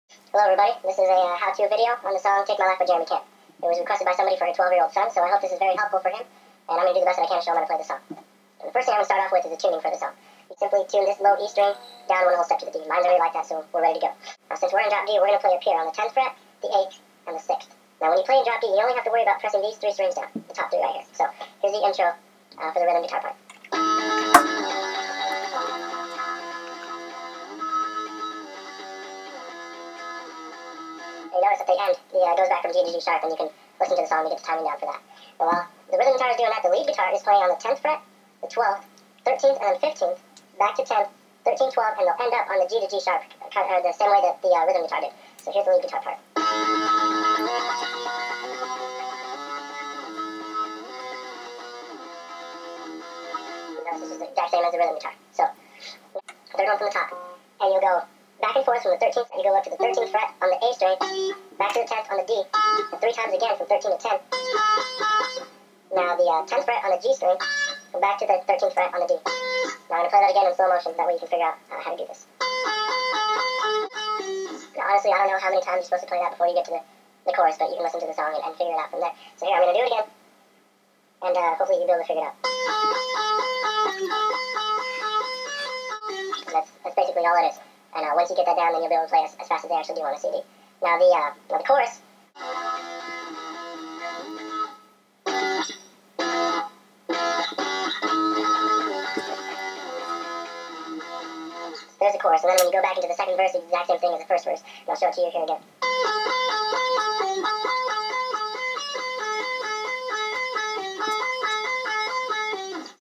Attempt at a [fake] guitar tutorial
(Sorry I haven't made any "real" videos in a while [or ever]. I know, I know, all of my vids are super lame lol. I don't have time to do much more than this these days haha) My friend made this guitar tutorial a while back, and I decided I would make one, too. I edited out parts of it just fyiiii

Come on, guys. It's really me talking in this video. Just accept it and move on.